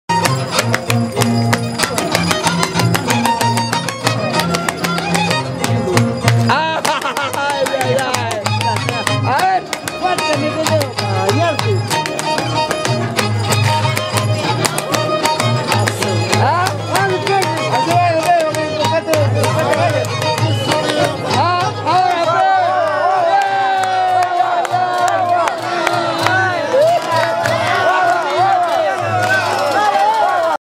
NAVIDAD AYACUCHO LARAMATE
comparsa de navidad del anexo de chalhuapuquio en  competencia en la plaza de laramate  frente ala comparsa de miraflores  . JHOMIRA PINO SERNA !! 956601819   .RPM #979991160